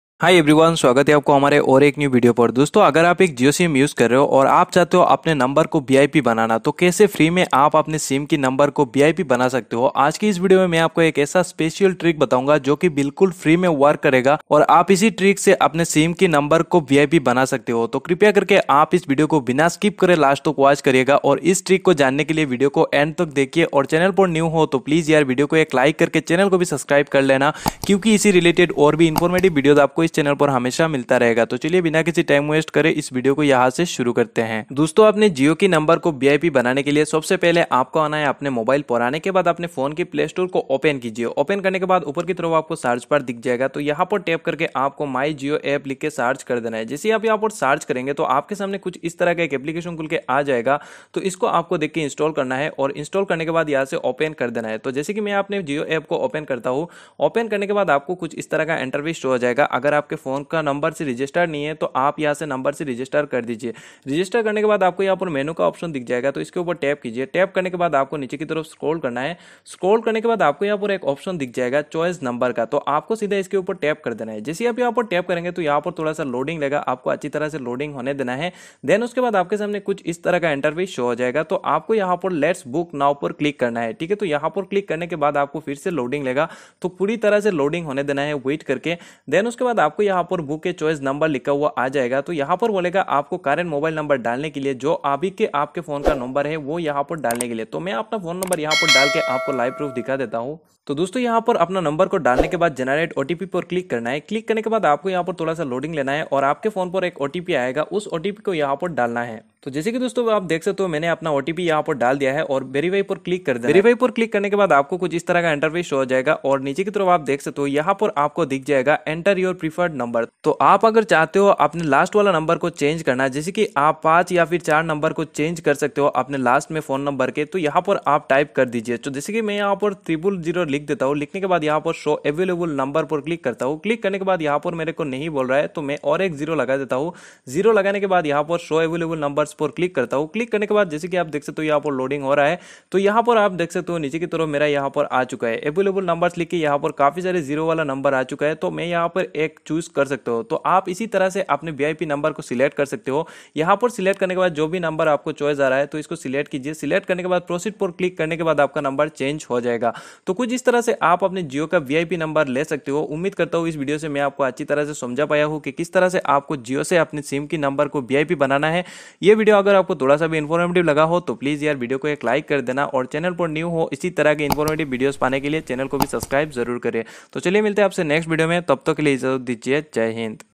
Jio VIP Number Kaise Le | How To Get Jio VIP Number | Online Vip Mobile Number Kaise Le
Hi Friends Is video main maine aapko bataya ki agar apka Jio sim ka number VIP bana na hai tho kaise ap ghar bete mobile se apne jio sim ka  number ko vip bana sakty hai